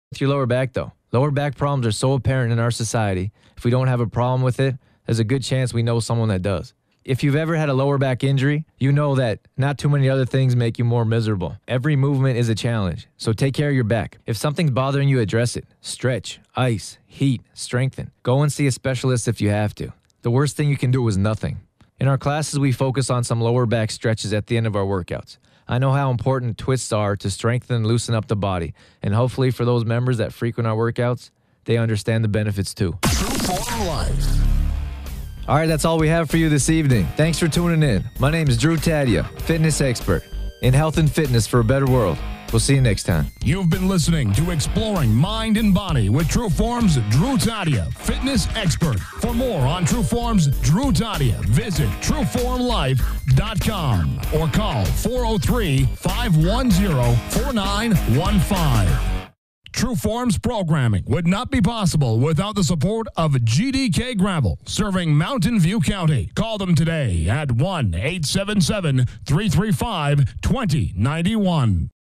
EPISODE #26 Part 2 Exploring Mind and Body
Part 2:  Interview with Lesley Winfield who lost 22lbs so far with True Form.  We're talking about health tips and lower back pain also.